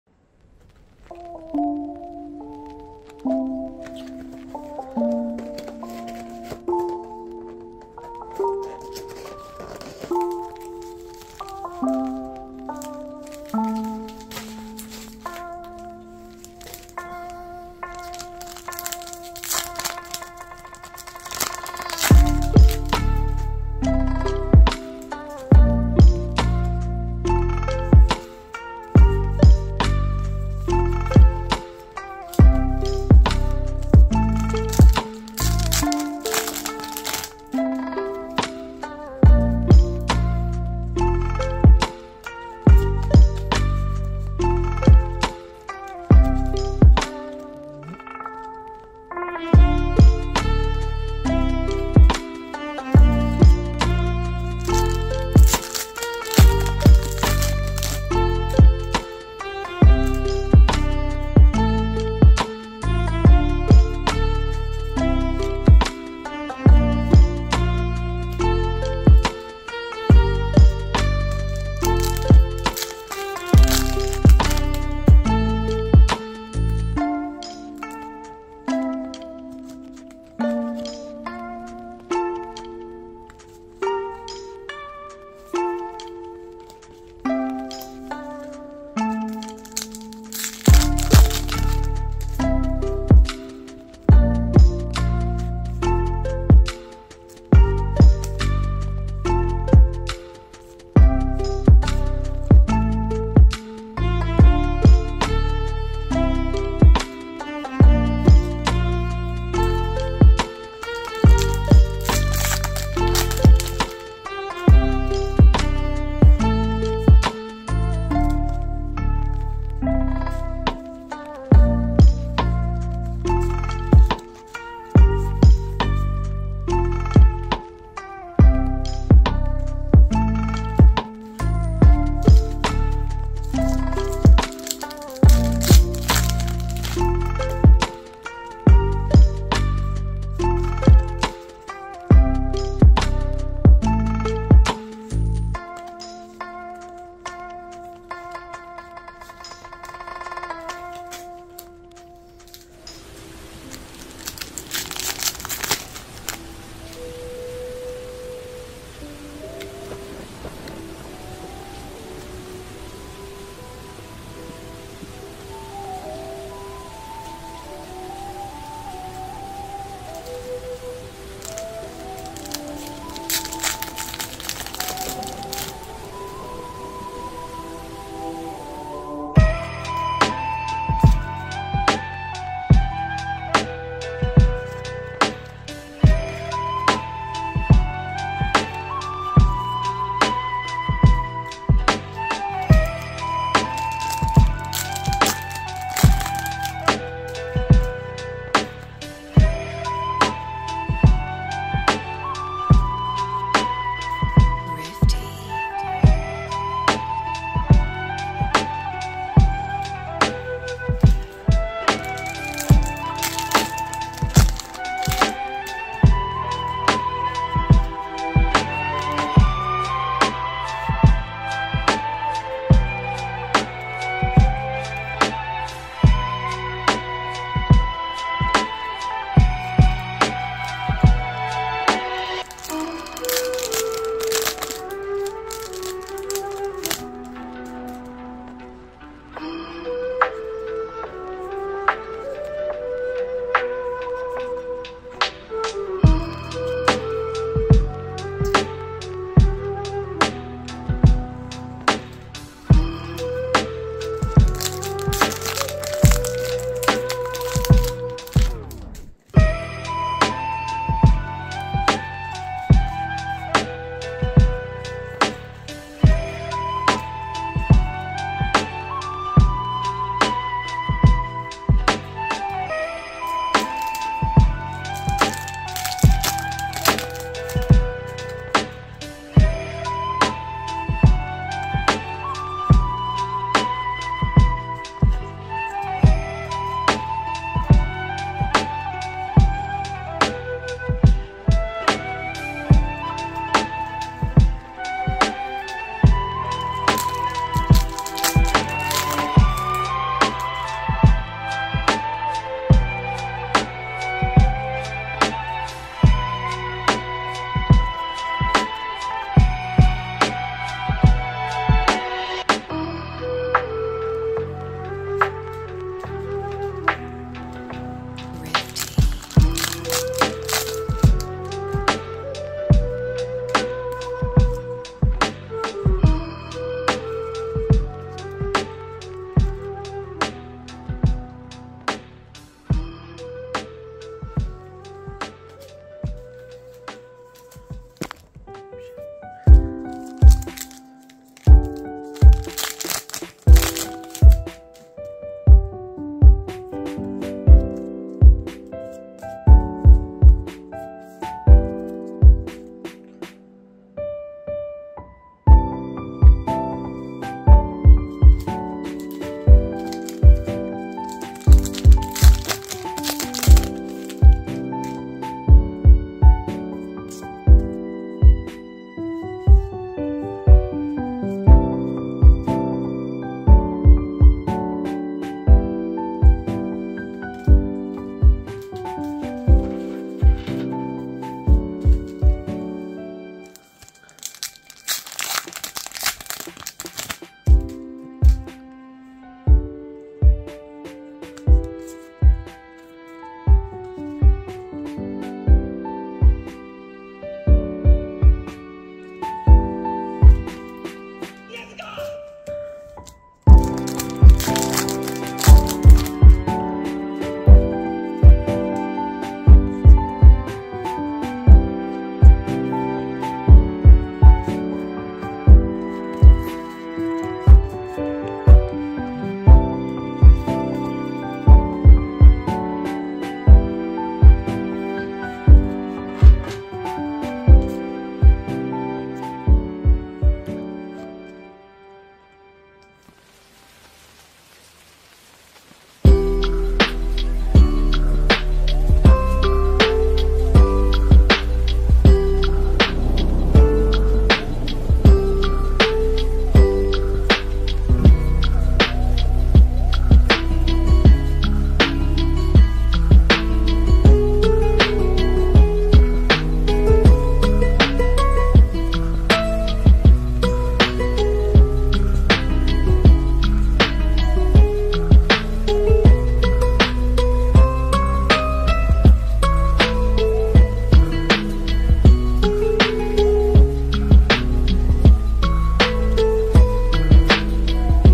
Card Expert - Rainbow Demon Slayer Booster Box Opening - Thanks Ishopanime!
It's my first time opening one of these Rainbow Demon Slayer Booster Boxes. Lets's see what we get!